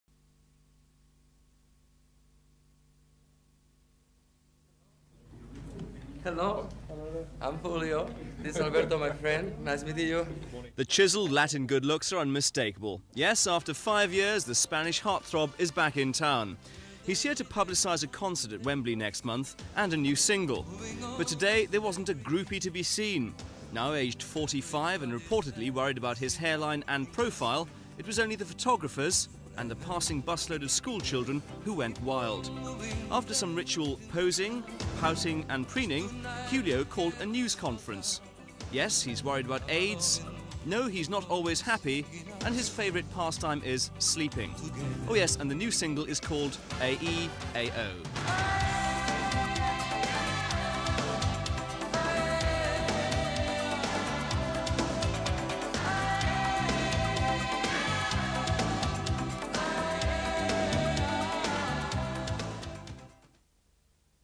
Julio Iglesias
Thames News following Julio Iglasias promoting his up and coming  single and concert in Wembley.